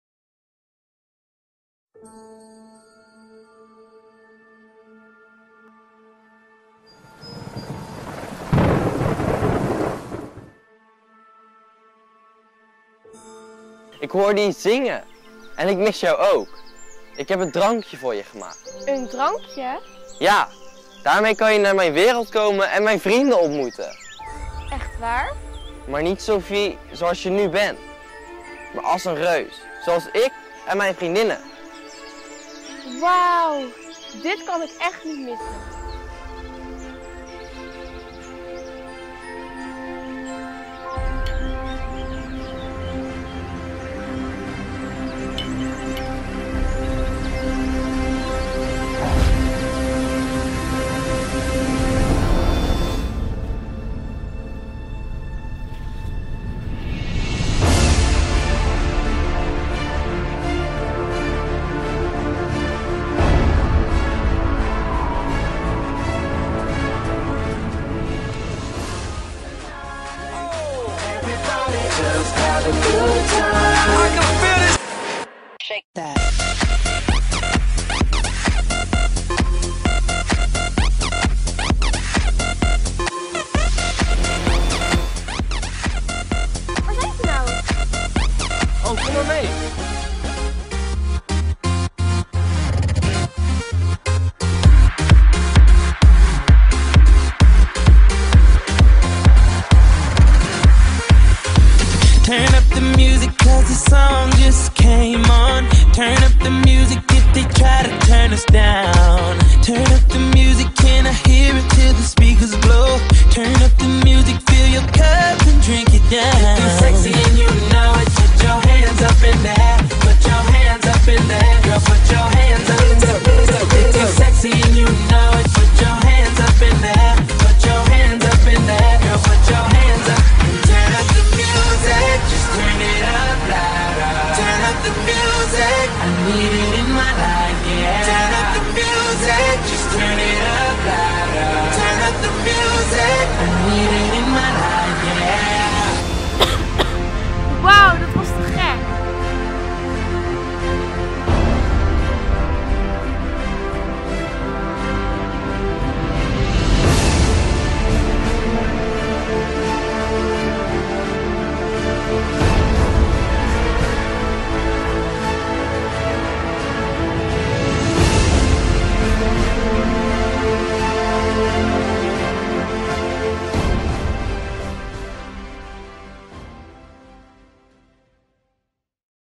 Grote vriendelijke reus NEHALENNIA
Tijdens de projecten marathon hebben ACDMI arts leerlingen een short movie gemaakt om geld in te zamelen voor Unicef.
Tijdens de voorstelling op 1december was deze movie het eind van het geheel.